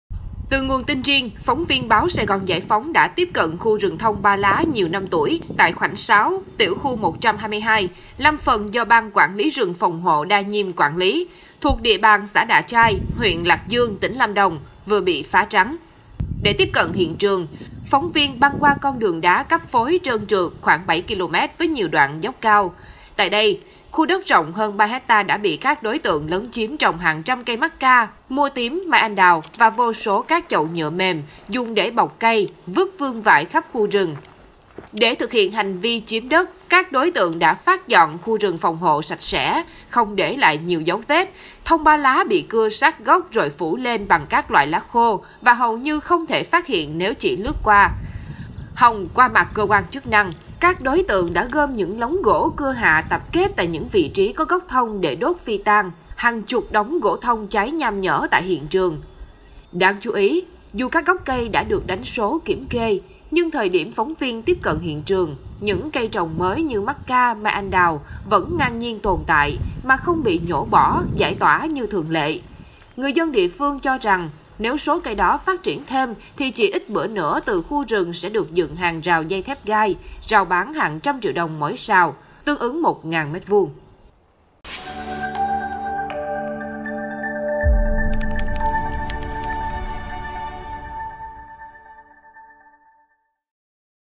Rừng phòng hộ Đạ Chais bị phá trắng, chiếm đất trồng mắc ca, mai anh đào
Phóng viên Báo SGGP đã tiếp cận khu rừng thông ba lá nhiều năm tuổi tại khoảnh 6, tiểu khu 122, lâm phần do Ban quản lý rừng phòng hộ Đa Nhim quản lý (thuộc địa bàn xã Đạ Chais, huyện Lạc Dương, tỉnh Lâm Đồng) vừa bị phá trắng.